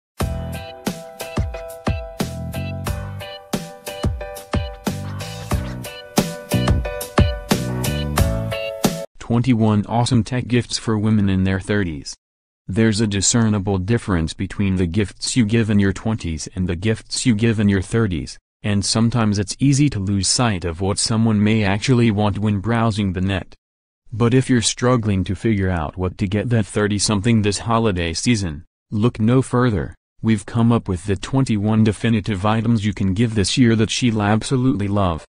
21 Awesome Tech Gifts For Women in Their 30s
21 Awesome Tech Gifts For Women in Their 30s.
There's a discernible difference between the gifts you give in your 20s and the gifts you give in your 30s — and sometimes it's easy to lose sight of what someone may actually want when browsing the net. But if you're struggling to figure out what to get that 30-something this holiday season, look no further: we've come up with the 21 definitive items you can give this year that she'll absolutely love.